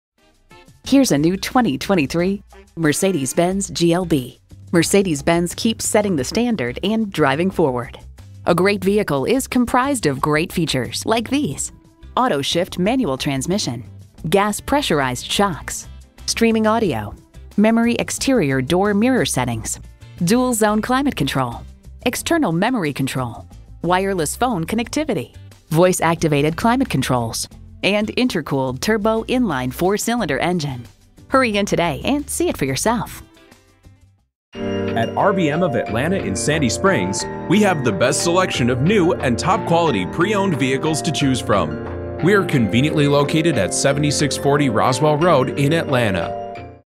New 2023 Mercedes-Benz GLB Atlanta GA Sandy Springs, GA G3934 - SOLD
Year: 2023
Make: Mercedes-Benz
Model: GLB
Trim: GLB 250 SUV
Engine: 4 2.0 L
Transmission: Automatic
Color: Polar White
Interior: Macchiato Beige
Stock #: G3934
VIN: W1N4M4GBXPW284144
WHEELS: 7.5J X 19 10-SPOKE W/MATTE BLACK ACCENTS -inc: Tires: 235/50R19, WHEEL LOCKING BOLTS, SPIRAL LOOK TRIM, SIRIUSXM RADIO, PREMIUM PACKAGE -inc: Hands-Free Access, Power-Folding Side Mirrors, 10.25 Digital Instrument Cluster, Auto-Dimming Rearview & Driver-Side Mirrors, Keyless-Go, 10.25 Media Display w/Touchscreen, 115V Outlet, POLAR WHITE, PARKING ASSISTANCE PACKAGE -inc: Active Parking Assist, Surround View System, PANORAMA ROOF, MACCHIATO BEIGE, MB-TEX UPHOLSTERY, HEATED STEERING WHEEL -inc: Non-Flattened Steering Wheel,. This Mercedes-Benz GLB has a dependable Intercooled Turbo Premium Unleaded I-4 2.0 L/121 engine powering this Automatic transmission.*This Mercedes-Benz GLB GLB 250 Has Everything You Want *FIRST AID KIT, BEIGE HEADLINER, 64-COLOR INTERIOR AMBIENT LIGHTING -inc: Front chrome air vents interior changes to chrome instead of black, Window Grid Antenna, Wheels: 7.5J x 18 Twin 5-Spoke, Voice Activated Dual Zone Front Automatic Air Conditioning, Turn-By-Turn Navigation Directions, Trunk/Hatch Auto-Latch, Trip Computer, Transmission: 8-Speed Dual Clutch (DCT) -inc: shift paddles and DYNAMIC SELECT, Tires: 235/55R18, Tire Specific Low Tire Pressure Warning, Systems Monitor, Strut Front Suspension w/Coil Springs, Smart Device Remote Engine Start, Side Impact Beams, Roof Rack Rails Only, Roll-Up Cargo Cover, Remote Releases -Inc: Power Cargo Access, Remote Keyless Entry w/Integrated Key Transmitter, 4 Door Curb/Courtesy, Illuminated Entry and Panic Button.* Visit Us Today *A short visit to RBM of Atlanta (South) located at 7640 Roswell Rd, Atlanta, GA 30350 can get you a reliable GLB today!

Address:
7640 Roswell Road